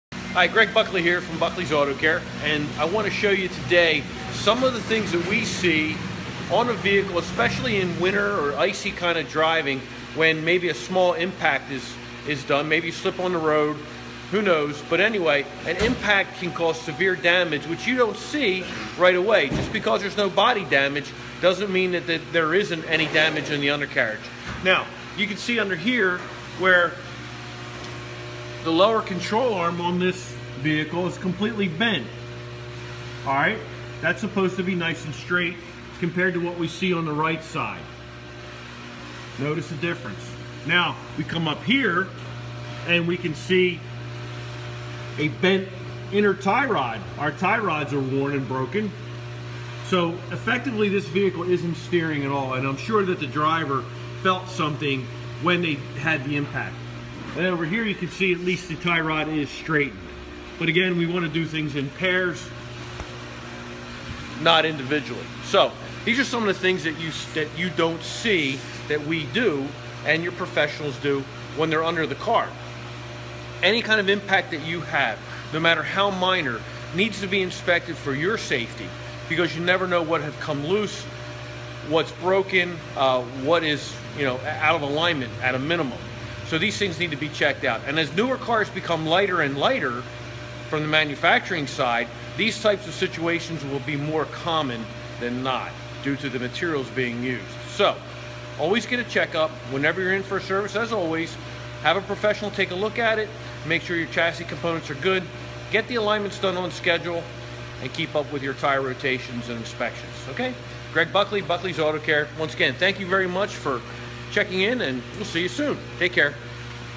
Ford Windstar Damaged Lower Control Arms
Here's a great sample of what can happen in the slightest of accidents. We have a left front wheel, that was impacted by a simple curb hit. The damage to the vehicle, A 2004 Ford Windstar, was pretty severe.

The left side lower control arm was completely bent, the inner tie rod was completely bent, and the hub bearing was also impacted and damaged.

In order to return the steering to normal, we had to update all of the pieces mentioned above. Of course, we did an alignment along with installing four brand-new tires and a rim.

A full inspection was performed, and fortunately no other damage was sustained in this minor impact.